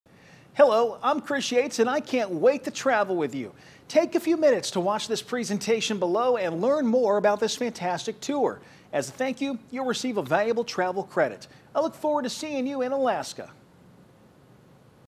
Alaska & Glacier Bay Cruise with Chris Yates
Join Chris Yates and fellow WMBD/WYZZ viewers for a 13-day guided vacation of our Alaska & Glacier Bay Cruise. Experience four nights aboard the luxurious Holland America’s ms Koningsdam ship, visit top bucket list attractions, and discover the breathtaking beauty that makes the Last Frontier a once-in-a-lifetime destination. Flights, accommodations, an expert Tour Director, and most meals are included.

Receive a $100 travel credit by watching the on-demand travel show for this tour.

Questions? Our team of expert Tour Consultants are happy to help.